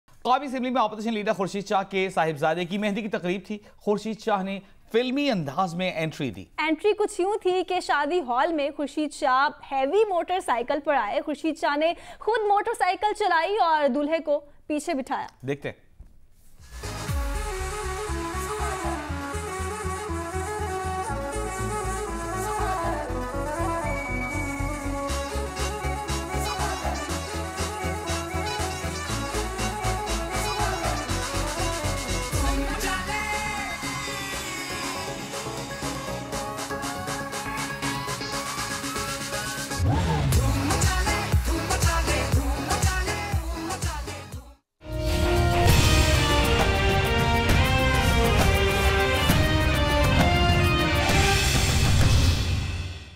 Khursheed Shah&#039;s entry at son&#039;s wedding a break from tradition
Special Report - Khursheed Shah&#039;s entry at son&#039;s wedding a break from tradition